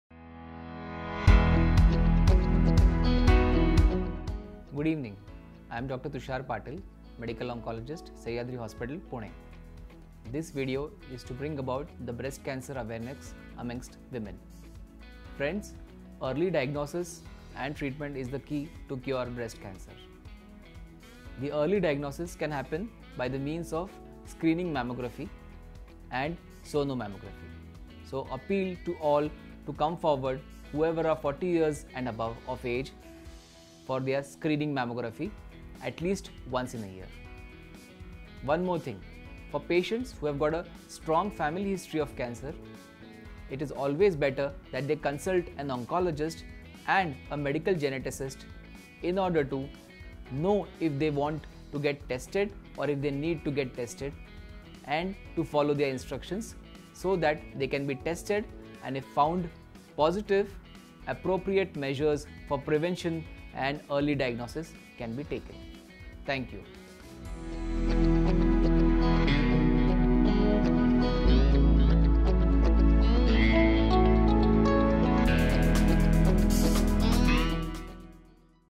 Breast Cancer Insights | Dr. Tushar Patil | MedGenome
MedGenome is dedicated to raising awareness about ways in which genetics can enhance your health and improve your life. With early detection and appropriate treatment, breast cancer can be cured completely!

Watch, Dr. Tushar Patil, Consultant Medical Oncologist, Sahyadri Hospitals, Pune explaining early detection of breast cancer and treatment including the role of genetic testing if you have a family history.

Genetic counseling can also help in the early diagnosis of breast cancer.